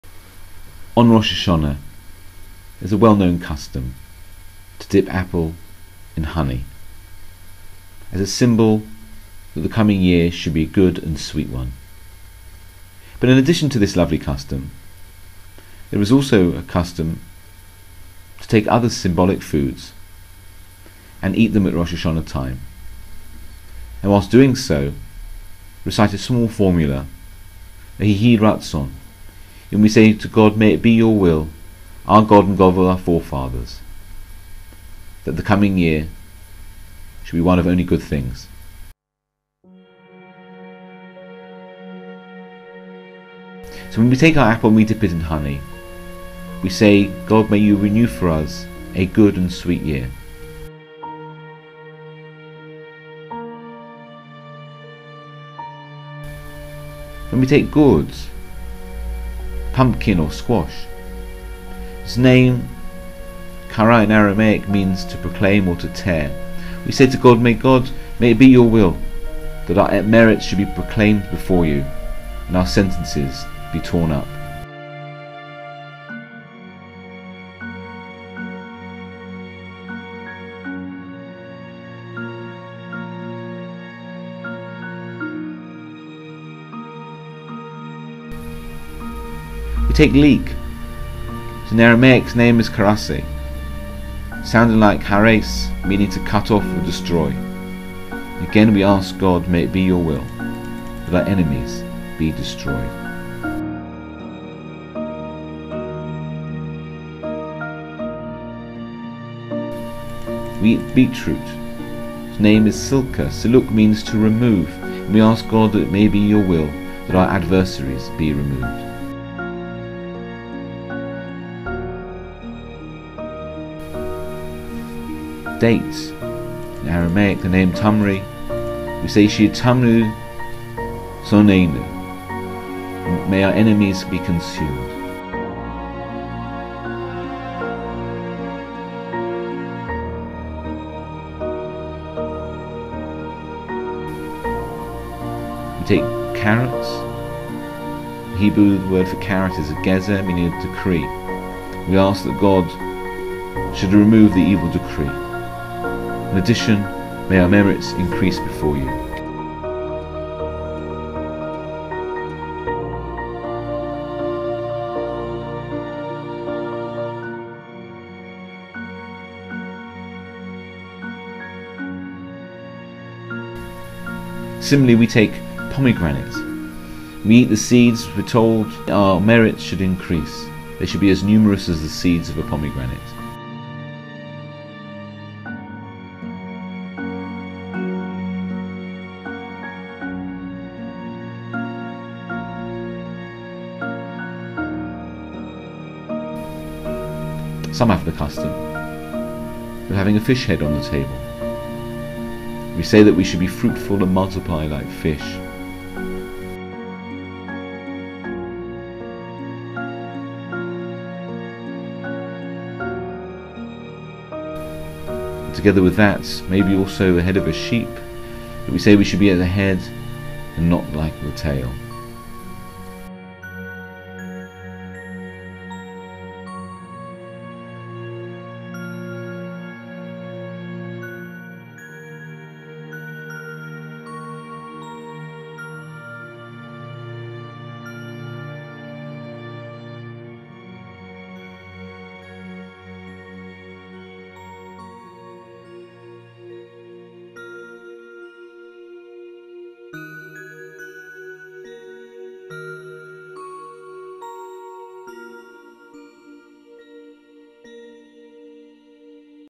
Simanim of Rosh Hashanah
Hey everyone! Rabbi Chapper wanted an animation for this rosh hashanah and here it is. Animated by rafs productions and voiced by Rabbi Chapper. I hope you enjoy and Shanah Tovah!!!